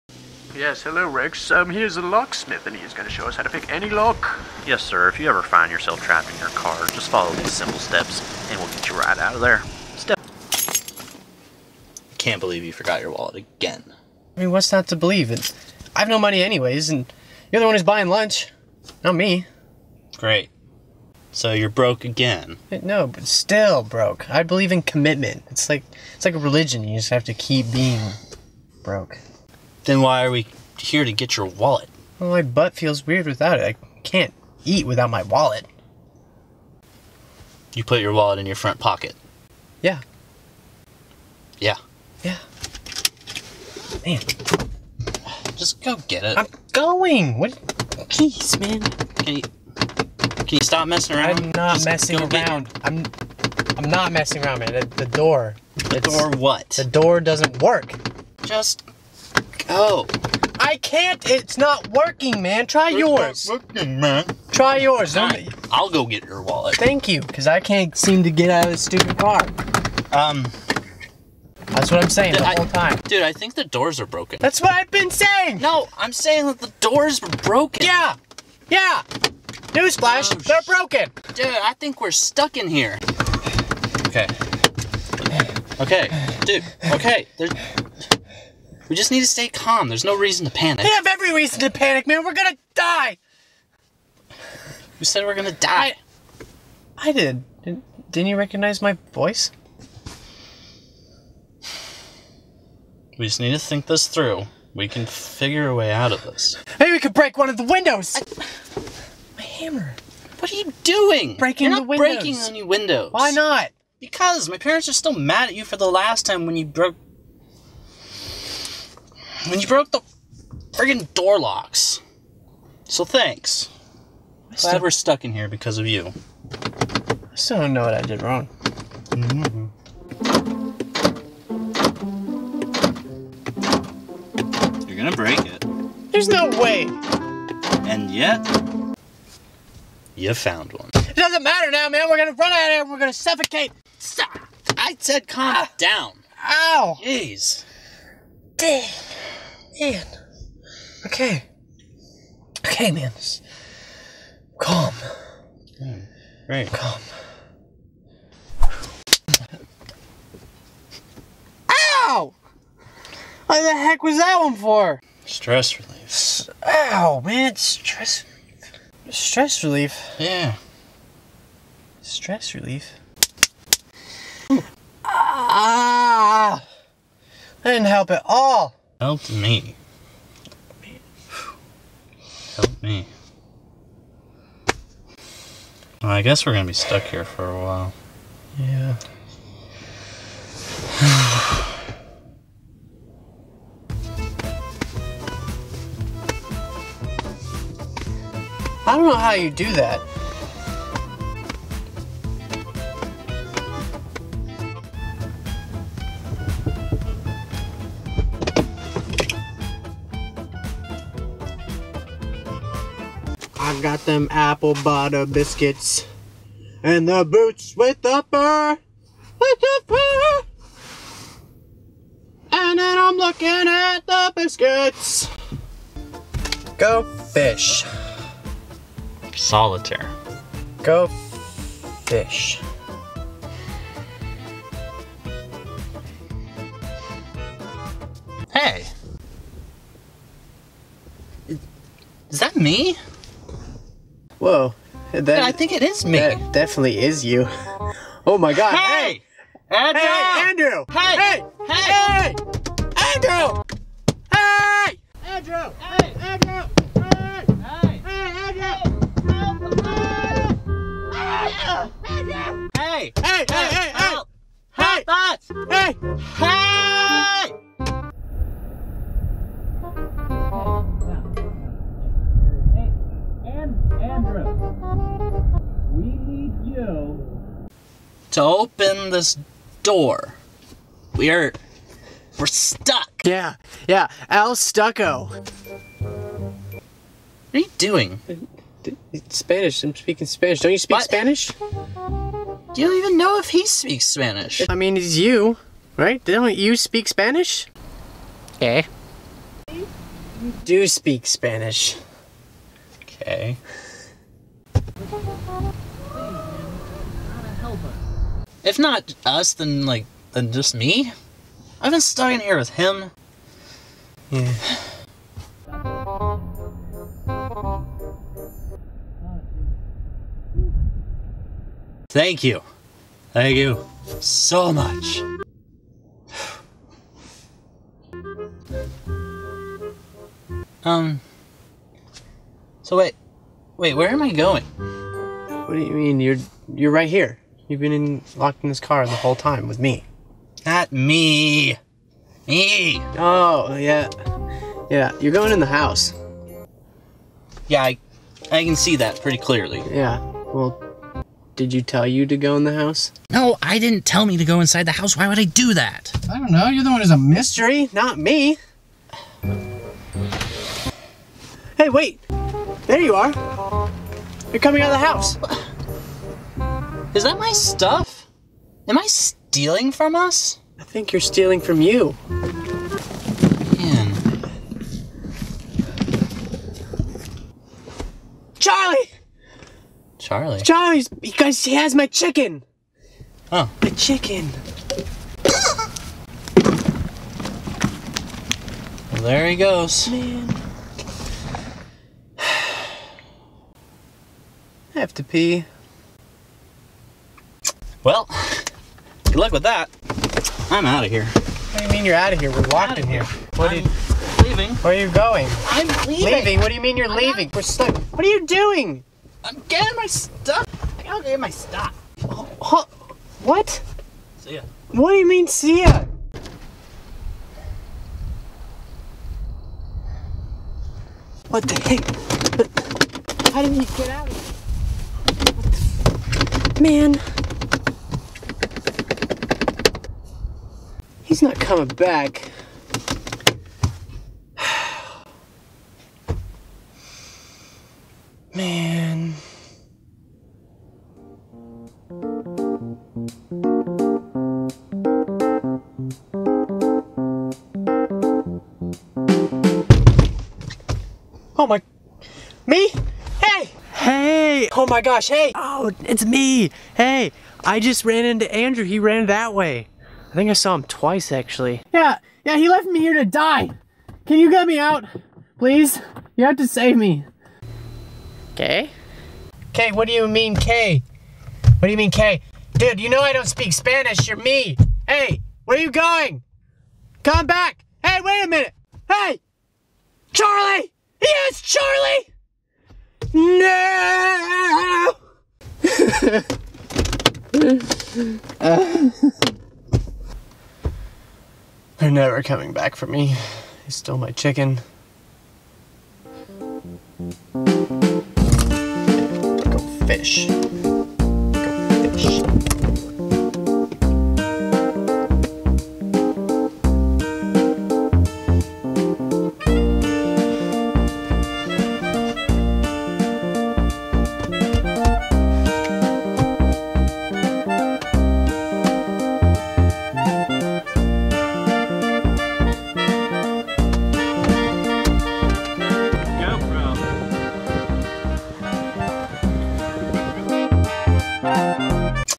Cartastrophe
Two guys are trapped inside a car! How will they escape? Who will save them?! Why are there clones in this video? So many questions, so many answers.

and